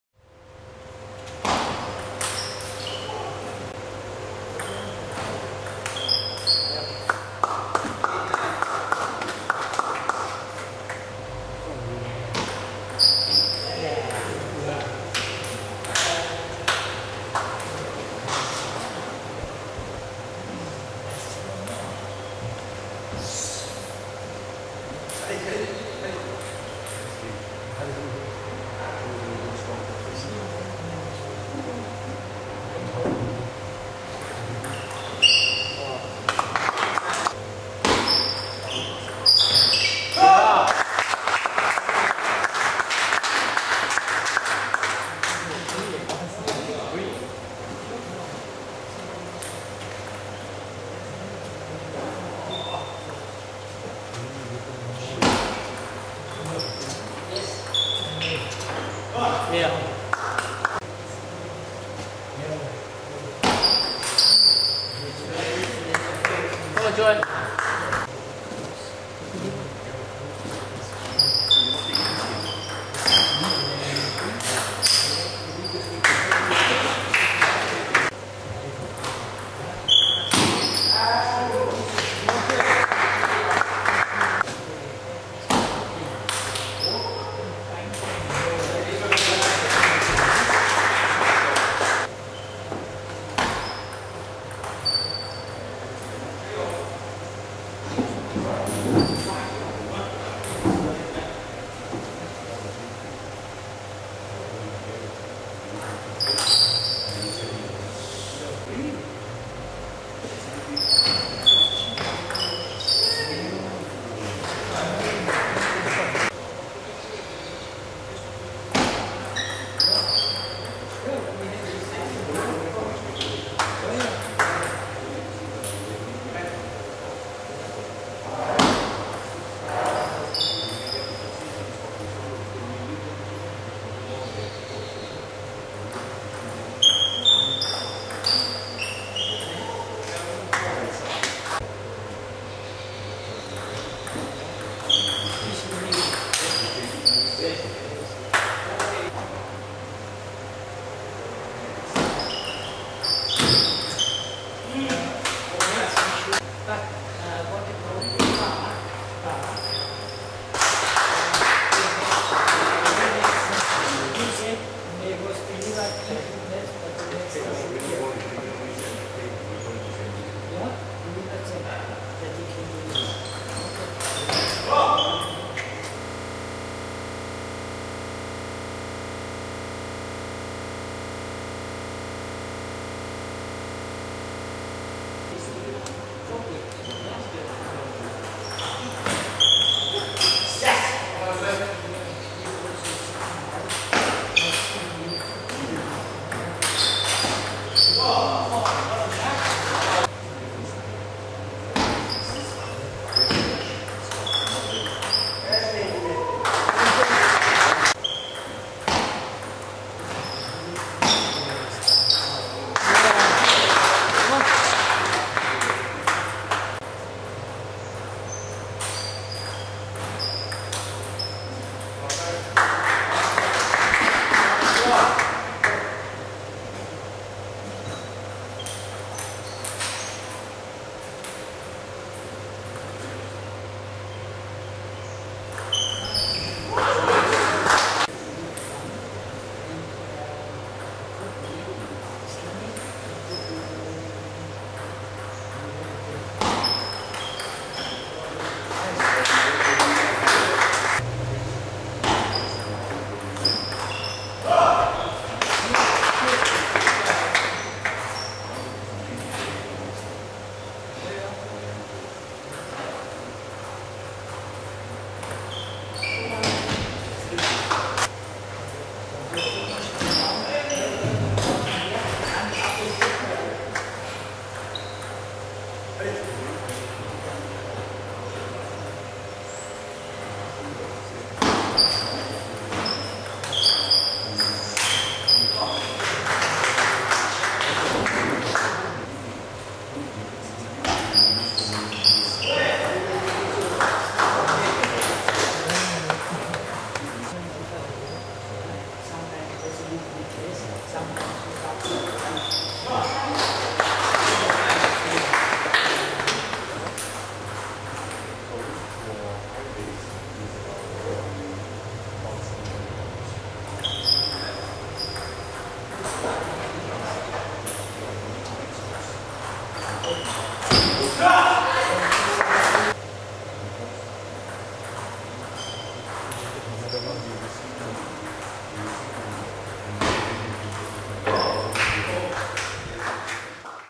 Choy Freddy vs Yoshua Shing
Table Tennis Fiji Open 2010, Winner: Choy Freddy/Kiribas